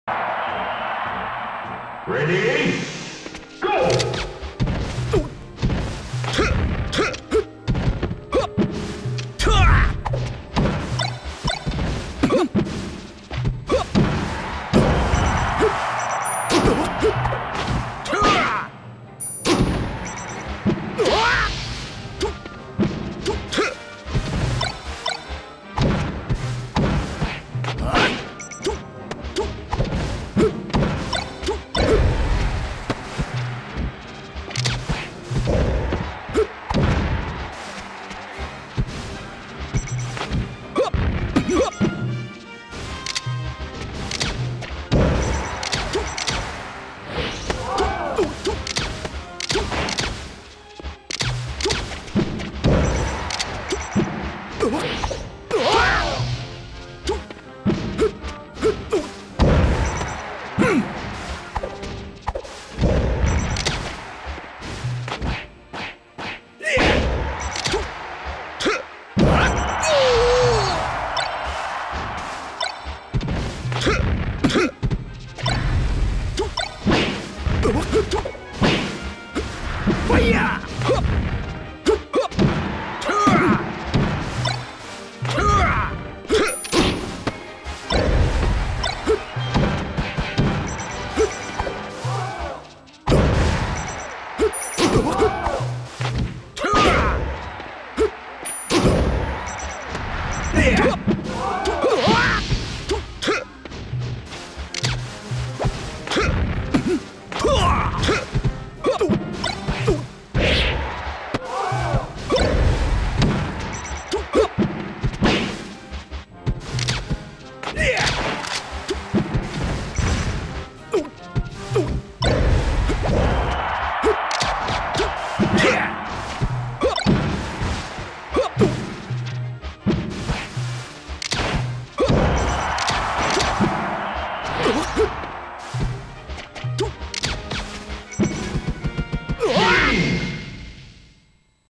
Friendlies | Joeri(Falco) vs Zgetto(Falcon) 1
Friendly best of 3's at Zgetto's place

13/07/2010